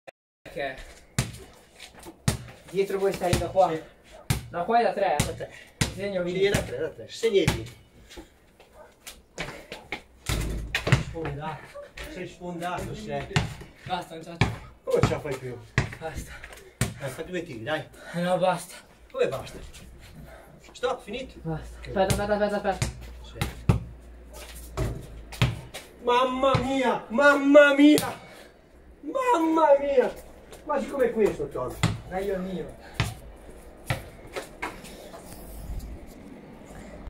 Incredibile canestro di piede ! Tommaso Olivieri GOALKEEPER
Tommy non sbaglia un colpo ! Che culooooo ! ( Tommaso Olivieri )